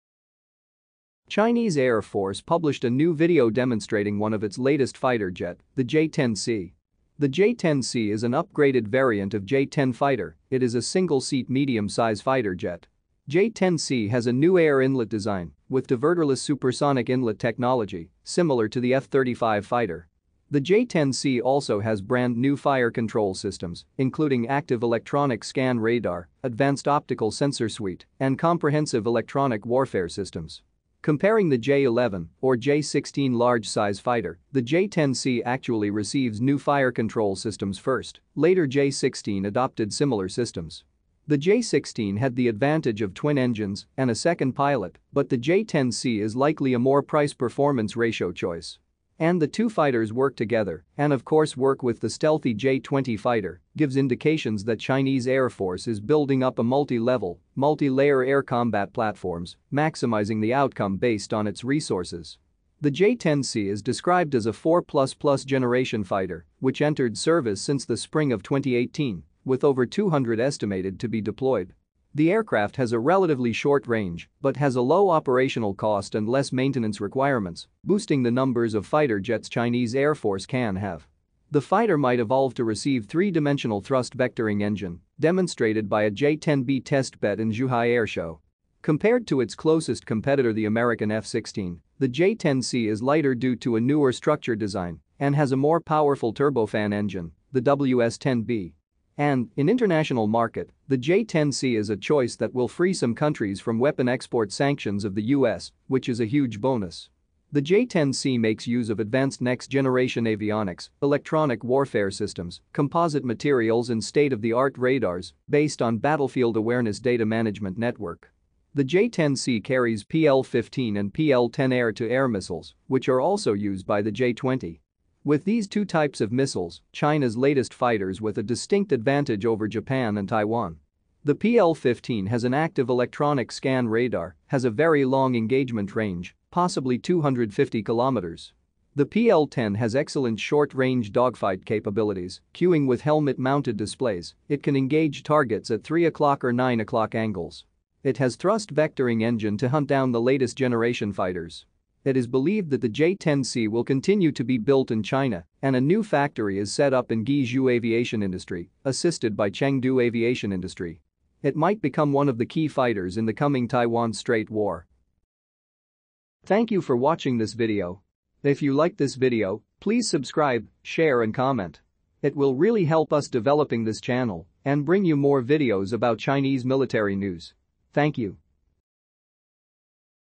Chinese J-10C beats F-16? Latest generation fighter packed with AESA radar and long range missile
Chinese Air Force published a new video demonstrating one of its latest fighter jet, the J-10C.

The J-10C is an upgraded variant of J-10 fighter, it is a single seat medium size fighter jet. J-10C has a new air inlet design, with diverterless supersonic inlet technology, similar to the F-35 fighter.

The J-10C also has brand new fire control systems, including active electronic scan radar, advanced optical sensor suite, and comprehensive electronic warfare systems.

Comparing the J-11, or J-16 large size fighter, the J-10C actually receives new fire control systems first, later J-16 adopted similar systems. The J-16 had the advantage of twin engines, and a second pilot, but the J-10C is likely a more price performance ratio choice. And the two fighters work together, and of course work with the stealthy J-20 fighter, gives indications that Chinese Air Force is building up a multi level, multi layer air combat platforms, maximizing the outcome based on its resources.

The J-10C is described as a 4 plus plus generation’ fighter, which entered service since the spring of 2018, with over 200 estimated to be deployed. The aircraft has a relatively short range, but has a low operational cost and less maintenance requirements, boosting the numbers of fighter jets Chinese Air Force can have.

The fighter might evolve to receive three dimensional thrust vectoring engine, demonstrated by a J-10B test bed in Zhuhai Air Show. Compared to its closest competitor the American F-16, the J-10C is lighter due to a newer structure design, and has a more powerful turbofan engine, the WS-10B.

And, in international market, the J-10C is a choice that will free some countries from weapon export sanctions of the U.S., which is a huge bonus.

The J-10C makes use of advanced next generation avionics, electronic warfare systems, composite materials and state of the art radars, based on battlefield awareness data management network.

The J-10C carries PL-15 and PL-10 air to air missiles, which are also used by the J-20. With these two types of missiles, China’s latest fighters with a distinct advantage over Japan and Taiwan. The PL-15 has an active electronic scan radar, has a very long engagement range, possibly 250km.

The PL-10 has excellent short range dogfight capabilities, cueing with helmet mounted displays, it can engage targets at 3 o’clock or 9 o’clock angles. It has thrust vectoring engine to hunt down the latest generation fighters.

It is believed that the J-10C will continue to be built in China, and a new factory is set up in Gui Zhou aviation industry, assisted by Cheng Du Aviation Industry. It might become one of the key fighters in the coming Taiwan Strait war.

Photo and video: Wiki common,